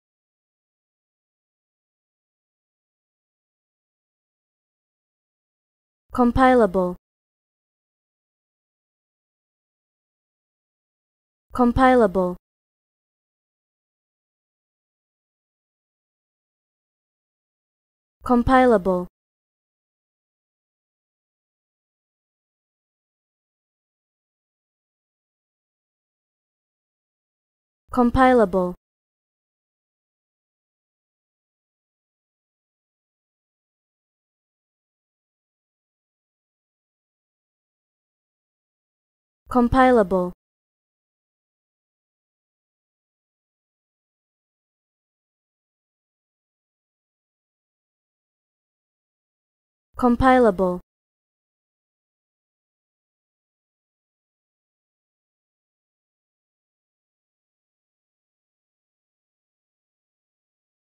How To Say Compilable
Learn how to say Compilable with EmmaSaying free pronunciation tutorials.